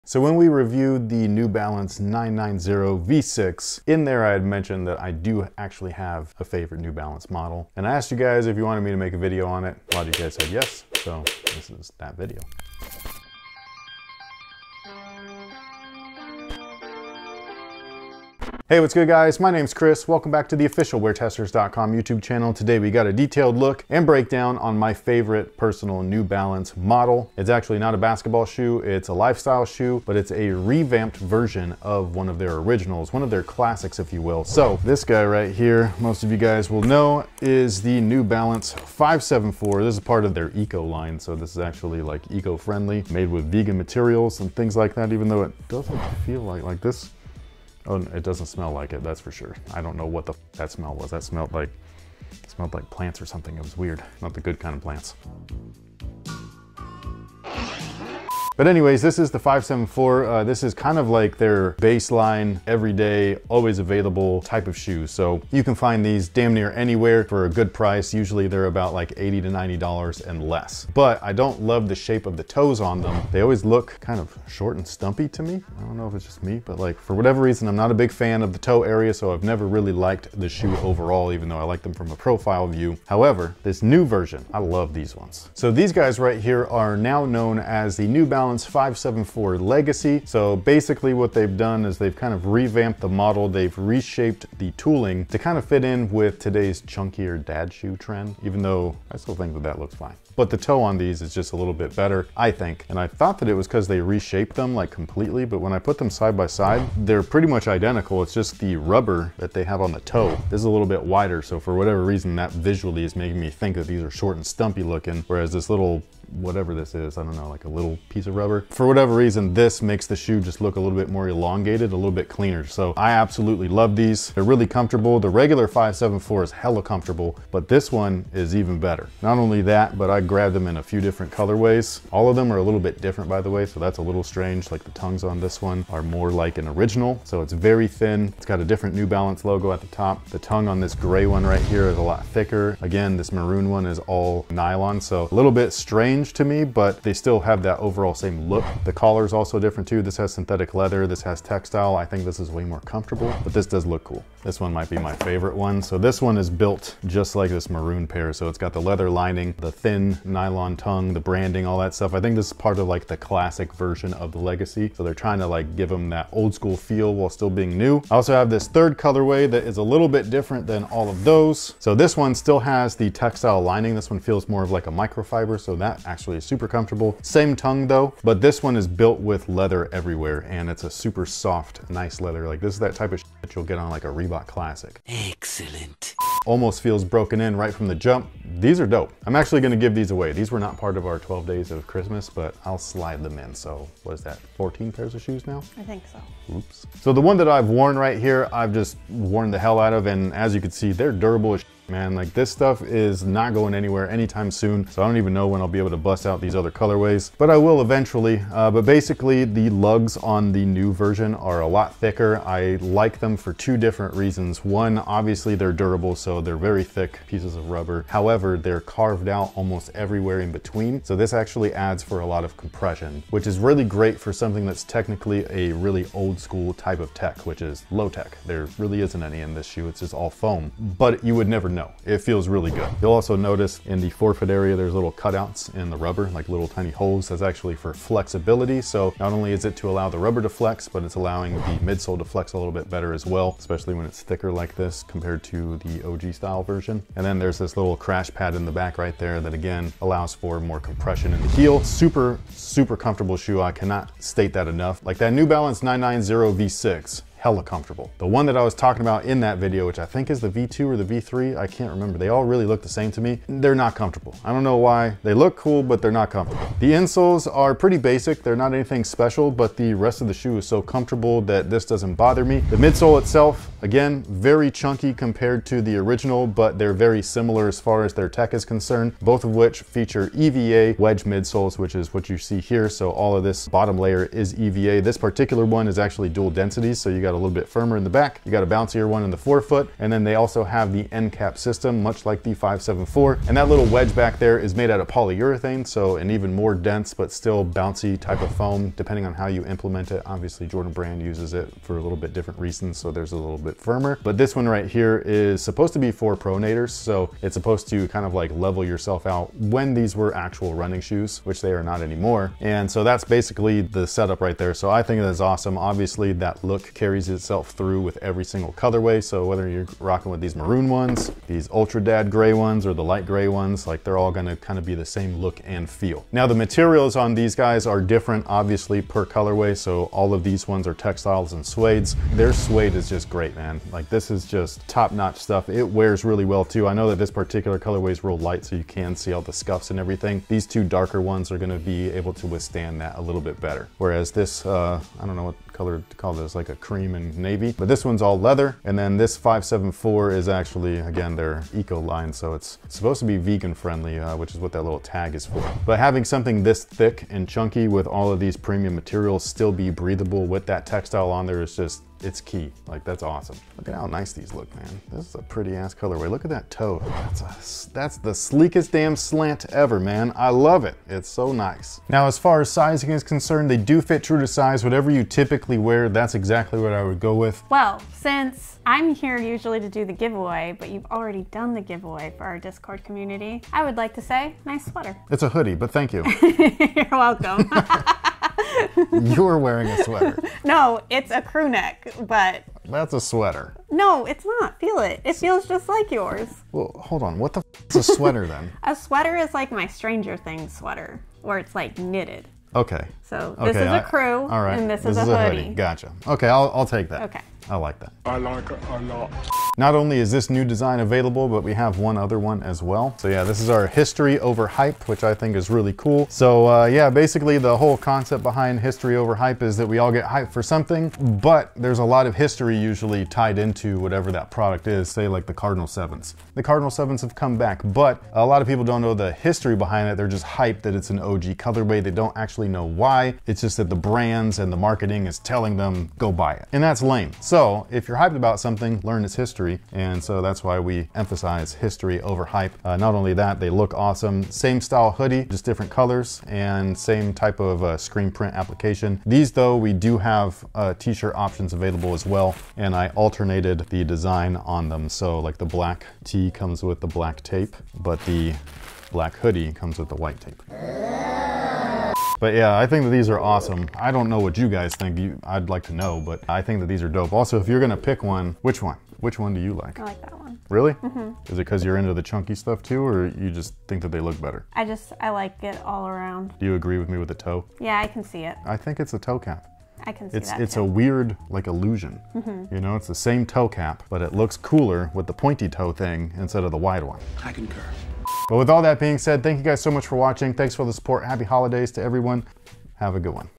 New Balance 574 Legacy: My Favorite New Balance Sneaker
Hey Guys! Today Chris talks about his favorite New Balance sneaker -- the New Balance 574 Legacy.